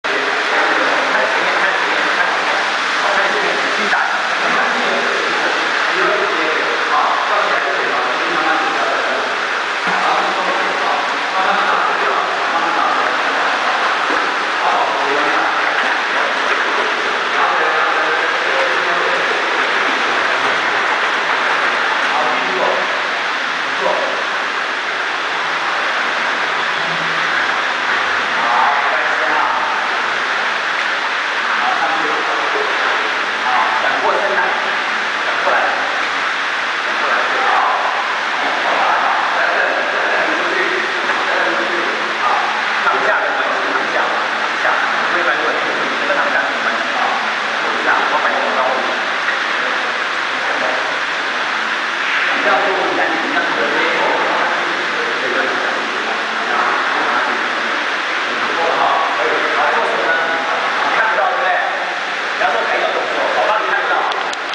2010.10.20-3自由式提腳正面與反面-1.MPG
一對一的游泳復健課程，由劉建明教練特訓